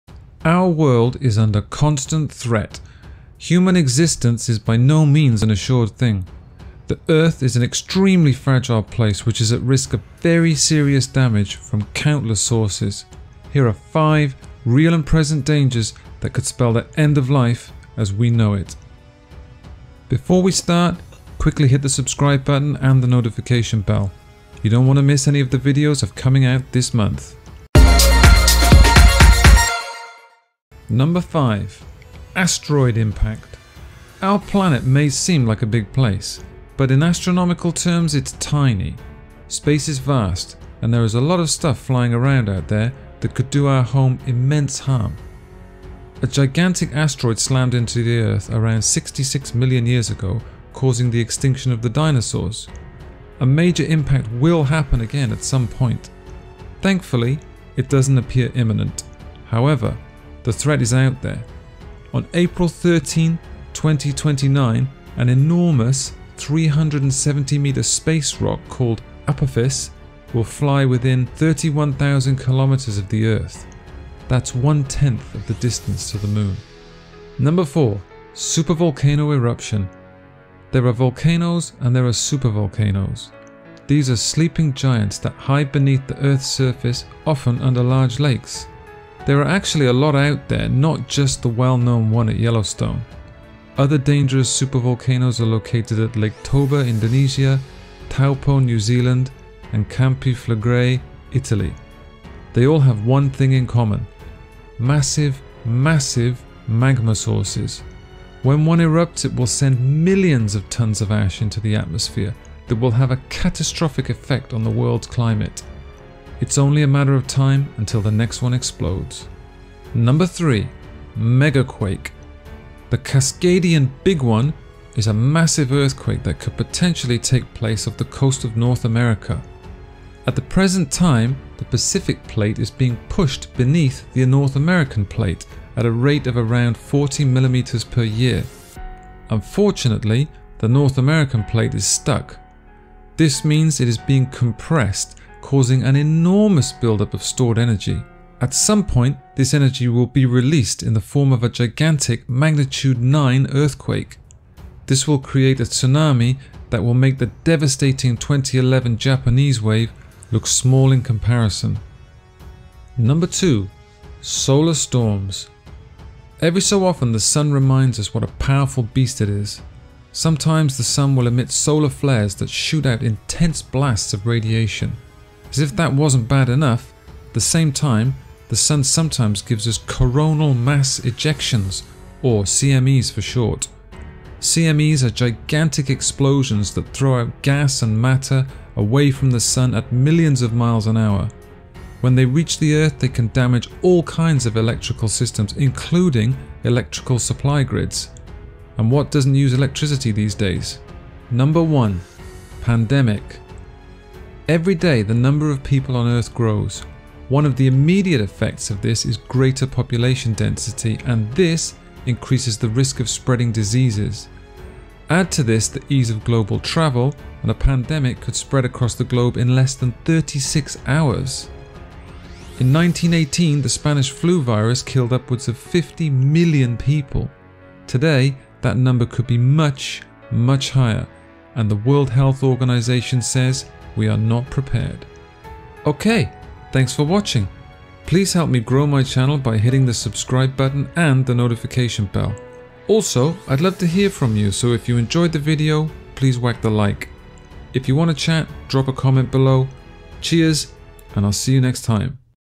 5 Natural Disasters Waiting To Happen
Scared for the future? You should be. They could all spell the endoftheworld as we know it. Our Earth is not a safe place and our future is far from assured.

There are dangerstotheworld everywhere. Protect yourself with the knowledge of what these naturaldisasters waiting to happen are!

The Earth has been here for billions of years, but humans have not. We've been on the planet for just a tiny tiny stretch of time. And that time could come to an end at any minute. We can't escape to another planet, despite what some people try and tell us!

If survive you'll have to be prepared. The first step is knowing the natural disasters waiting to happen.